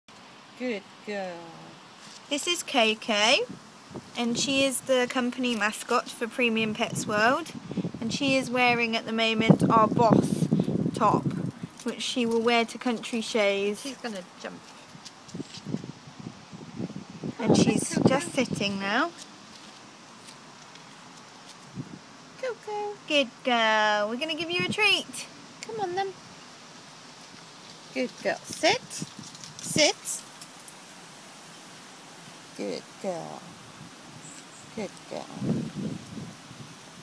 The Boss T-shirt modelled on my Blue Frenchie Coco
Premium Pets World Boss T-shirt available now, modelled on my blue frenchbulldog Coco.